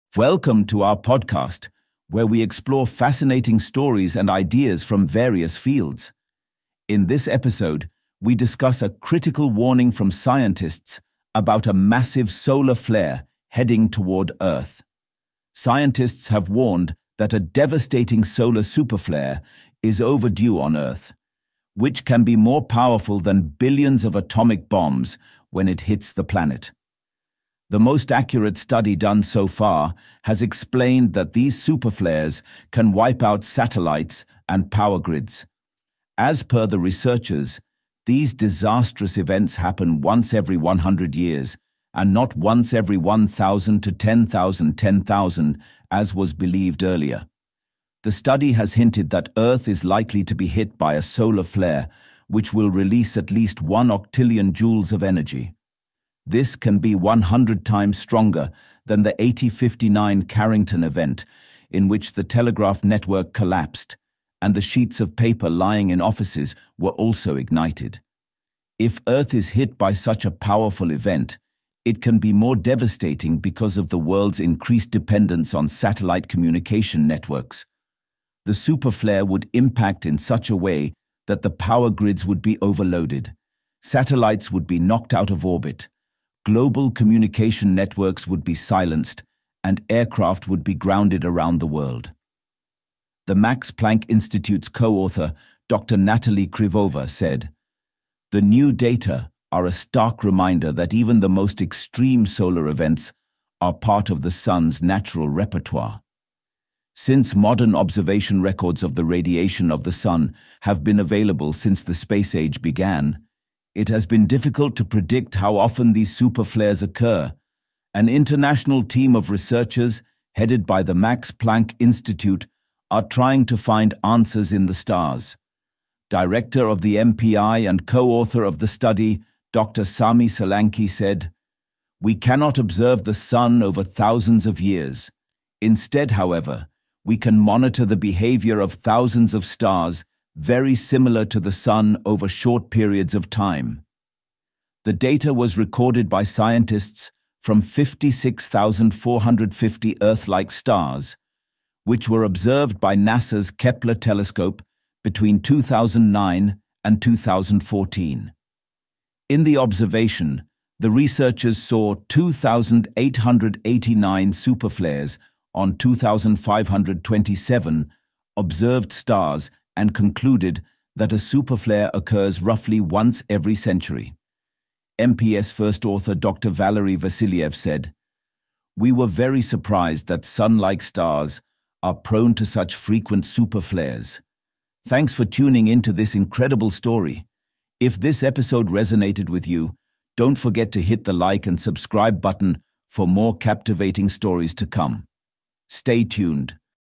Scientists Warn of Overdue Solar Superflare Threatening Earth’s Infrastructure | WION Podcast
Researchers have revealed that such superflares, once thought to occur only once every 1,000 to 10,000 years, actually happen once every 100 years. These powerful events could release energy 100 times stronger than the Carrington Event of 1859, potentially knocking out satellites, power grids, and global communication networks. The study, led by the Max Planck Institute, uses data from thousands of sun-like stars to predict the frequency of these dangerous solar events.